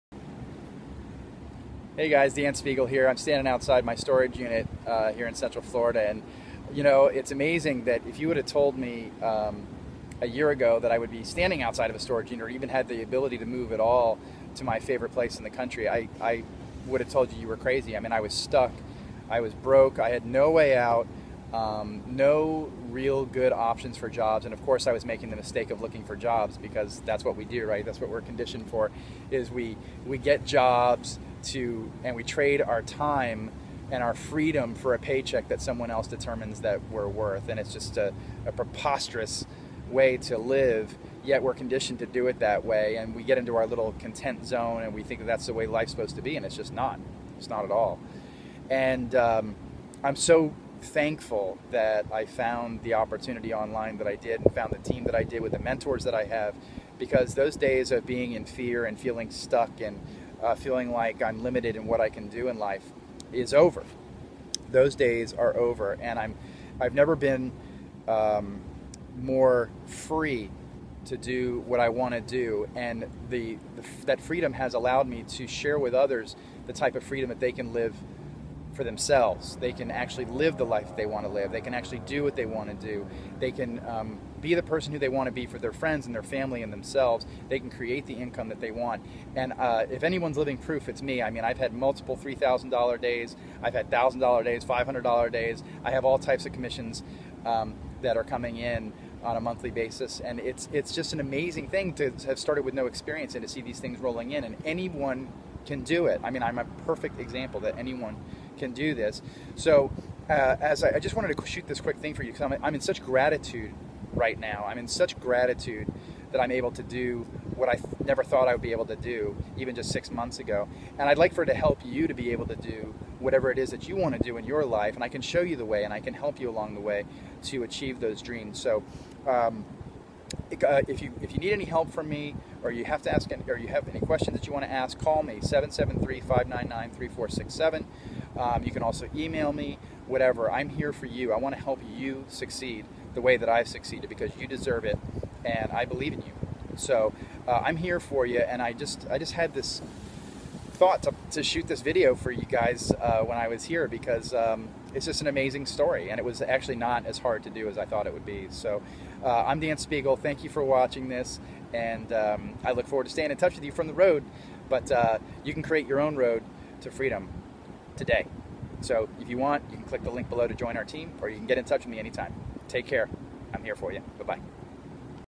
Storage Unit Thoughts
It is in a great state of gratitude that I am blessed to be moving to my favorite part of the country.  Had some quick thoughts so wanted to share!

Dan Spiegel...